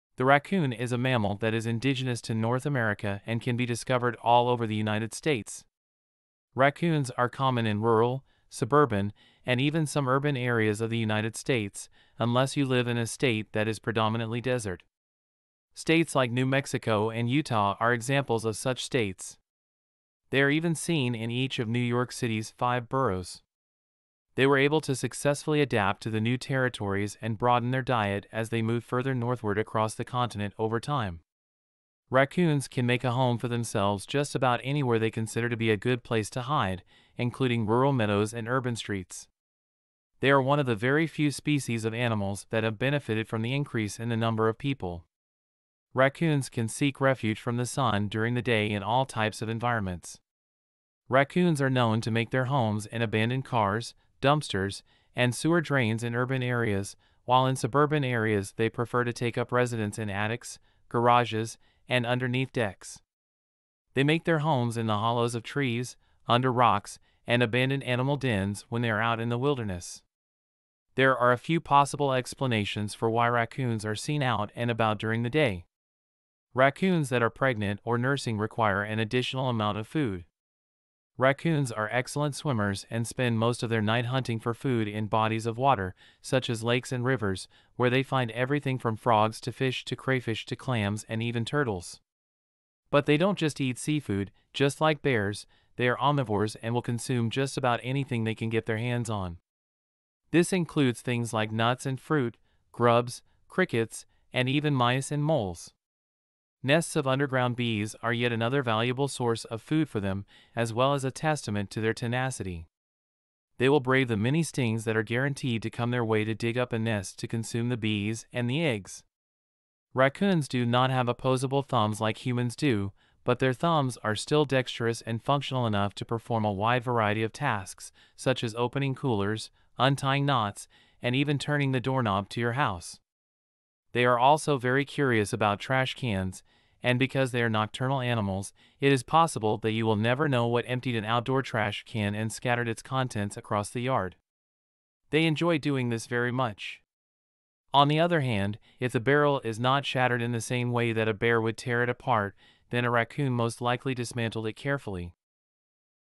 【Camojojo】Raccoons: Everything You Need to Know
Today's video shows how raccoons live in both cities and the wild. They really are omnivores because they eat many different kinds of food. They steal food from garbage cans in cities and can find many different kinds of food in the wild.